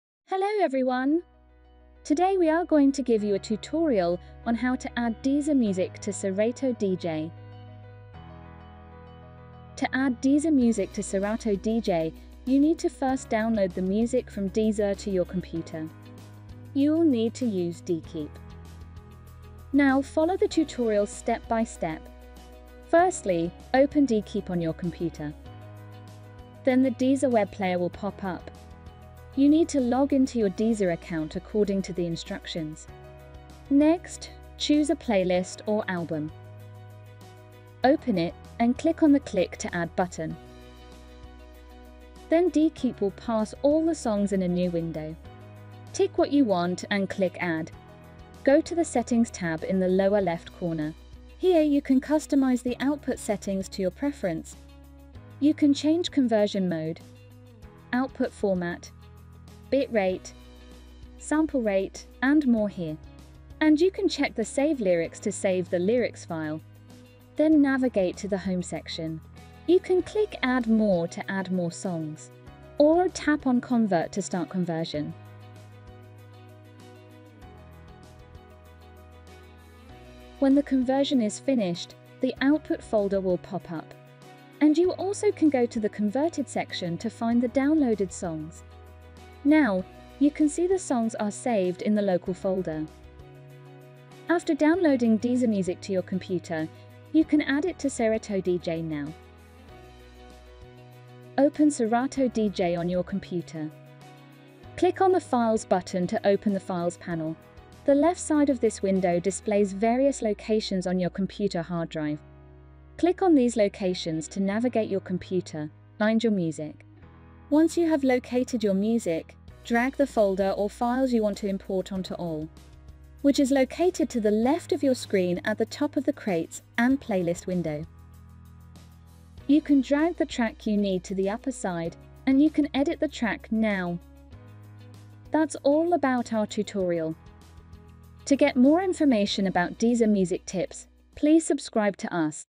[Solved!!!] How to Add Deezer Music to Serato DJ
In this video, we will give you a tutorial on how to add Deezer music to Serato DJ. To learn more about DeeKeep, please head

📝Brief Guide:
Step 1: Open DeeKeep on your computer and log into your Deezer account.
Step 2: Choose a playlist or album and click on the "Click to add" button.
Step 3: Tick what you want and click "Add".
Step 4: Go to "Settings" to customize the output settings to your preference.
Step 5: Navigate to "Home" and click "Convert" to start conversion.
Step 6: Find the converted songs in the "Converted" section or local folder.
Step 7: Open Serato DJ and click "Files" to add downloaded Deezer music to Serato DJ.